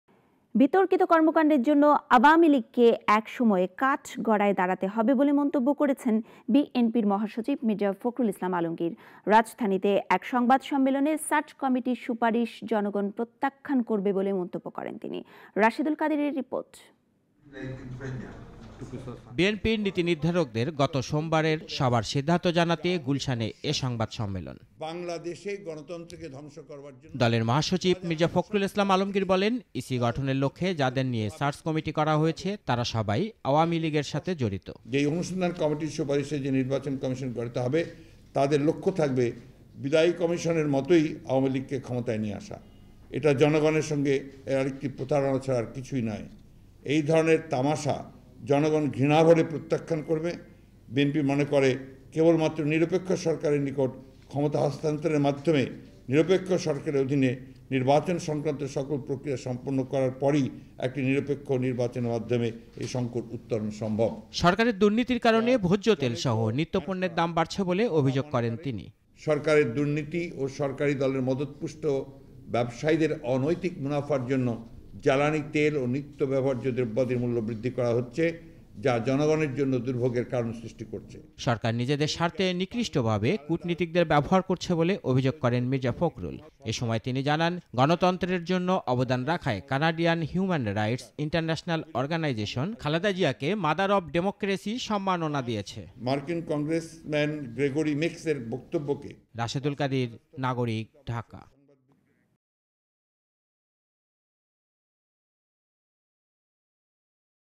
আ.লীগকে একসময় কাঠগড়ায় দাঁড়াতে হবে: মির্জা ফখরুল | BNP | Awami League | Mirza Fakhrul | Nagorik
বিতর্কিত কর্মকাণ্ডের জন্য আওয়ামী লীগকে (Awami League ) একসময় কাঠগড়ায় দাঁড়াতে হবে বলে মন্তব্য করেছেন বিএনপি মহাসচিব মির্জা ফখরুল ইসলাম আলমগীর। রাজধানীতে এক সংবাদ সম্মেলনে, সার্চ কমিটির সুপারিশ জনগণ প্রত্যাখ্যান করবে বলে মন্তব্য করেন তিনি।